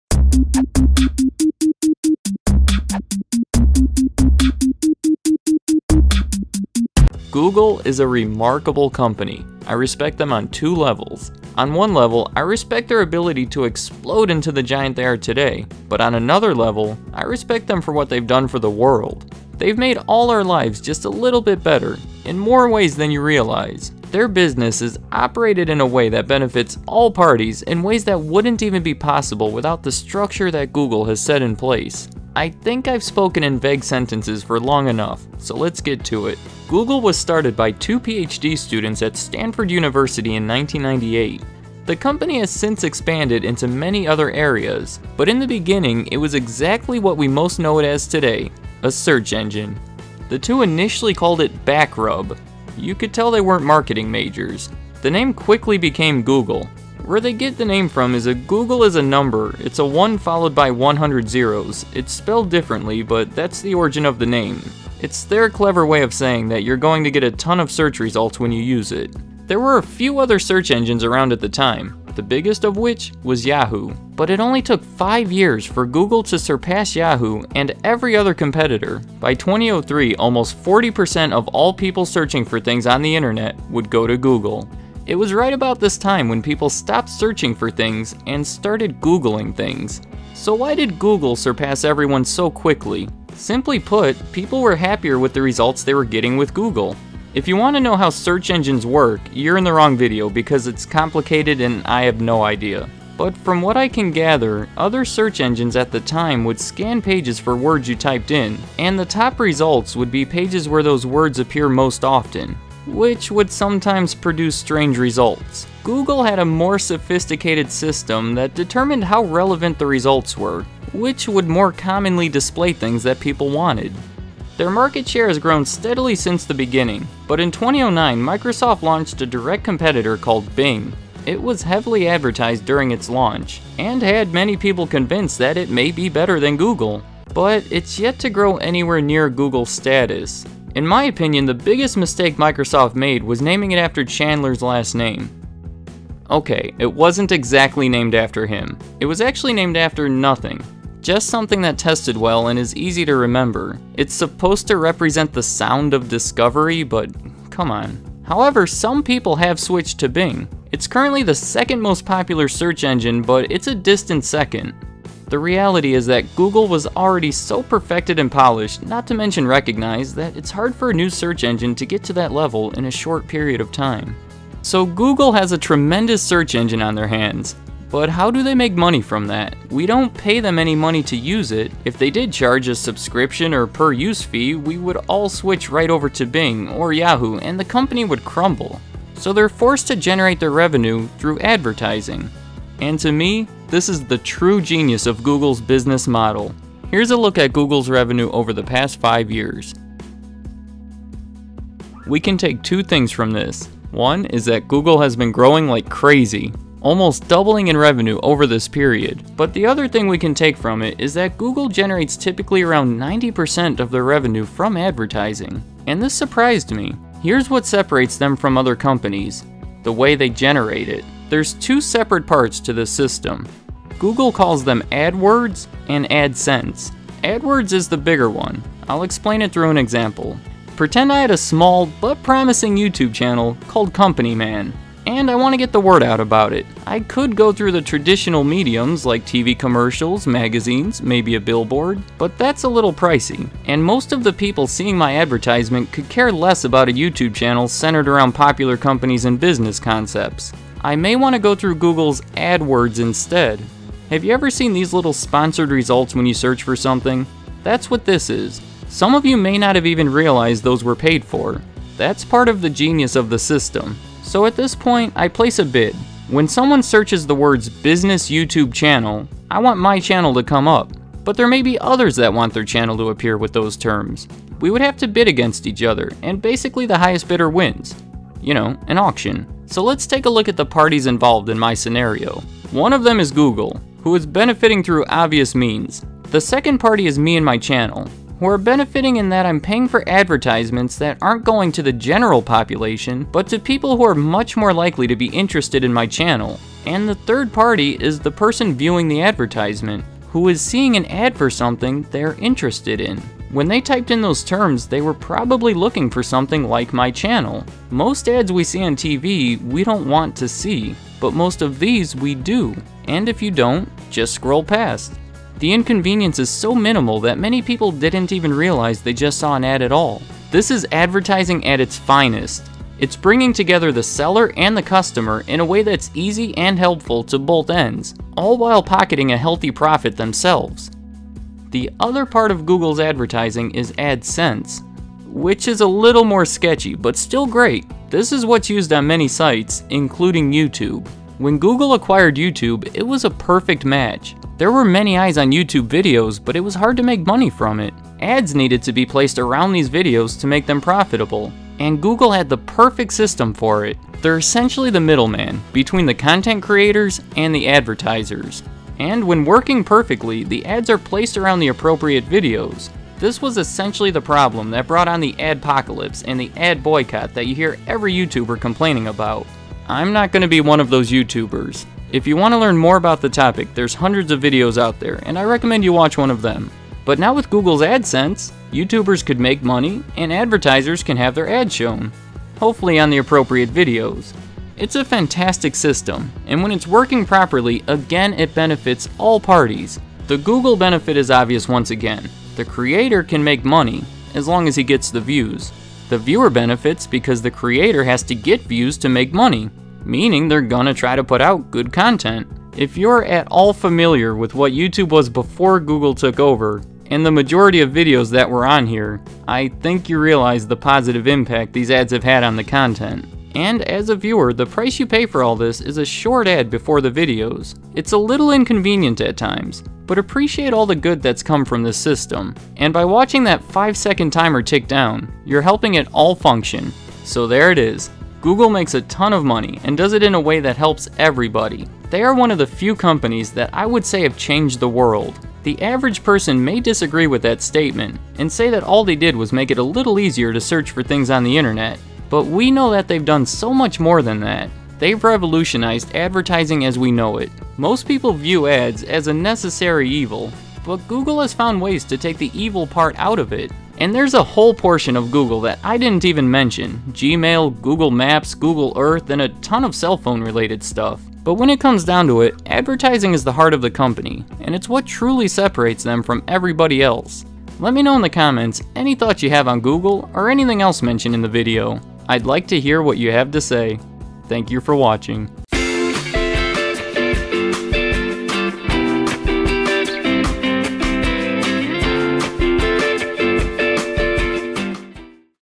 Google Is a Remarkable Company
We're all familiar with Google, but few are familiar with the extent of the impact they've made on the world. This video covers the origin of the company, how they make their money, and how they've innovated advertising as we know it. Google is truly a remarkable company.